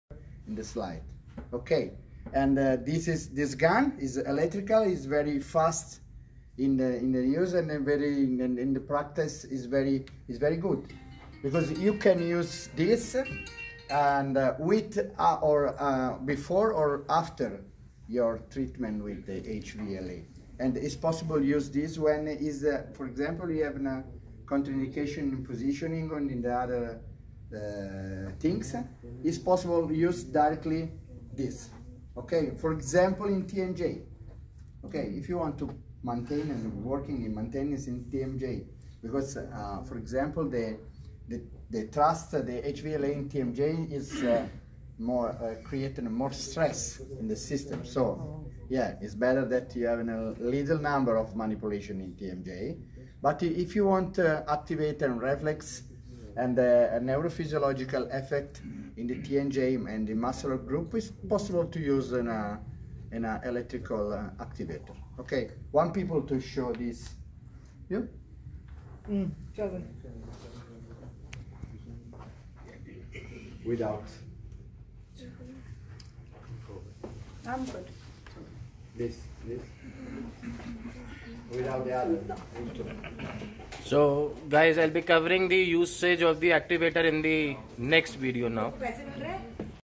CHIROPRACTIC GUN usage by Dr Giovanni . for APPOINTMENT at NOIDA.
In this video, Dr Vishwas Virmani is with Dr Giovanni Bonfanti ITALIAN OSTEOPATH at Ortho Neuro Chiropractic Physiotherapy Clinic .

ORTHO NEURO CHIROPRACTIC PHYSIOTHERAPY CLINIC
Bone/Joint/Nerve/Muscle PAINS/ACHES.

Category
Education
Category
Education